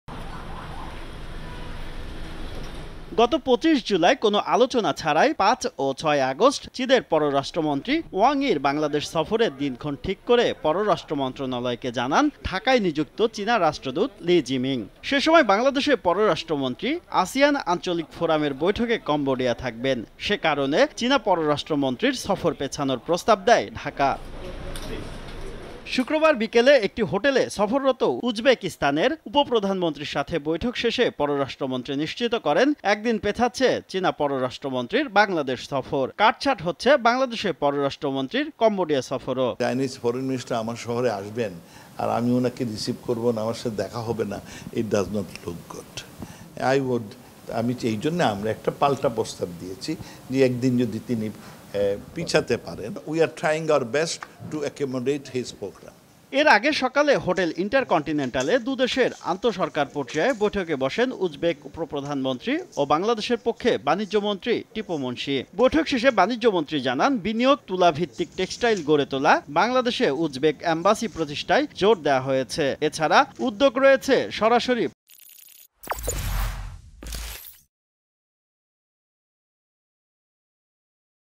বছরের শেষ দিকে ডলারের সংকট কমে আসবে: সালমান এফ রহমান | AK Abdul Momen | Salman F Rahman | Somoy TV
একদিন পেছালো চীনা পররাষ্ট্রমন্ত্রীর ঢাকা সফর। বিষয়টি নিশ্চিত করেছেন পররাষ্ট্রমন্ত্রী। বাংলাদেশের সঙ্গে আলোচনা না করে চীনা পররাষ্ট্রমন্ত্রীর ঢাকা সফরের তারিখ দেয়ায় এই জটিলতার সৃষ্টি হয়। এদিকে ঢাকায় ব্যস্ত সময় পার করছেন উজবেক উপপ্রধানমন্ত্রী নেতৃত্বে আসা প্রতিনিধি দল। বৈঠক করেছেন সরকারের দুই মন্ত্রী ও উপদেষ্টার সঙ্গে। বৈঠকে বাংলাদেশে উজবেক অ্যাম্বাসি প্রতিষ্ঠা, সরাসরি ফ্লাইট চালু,বিনিয়োগসহ বিভিন্ন বিষয় নিয়ে আলোচনা হয়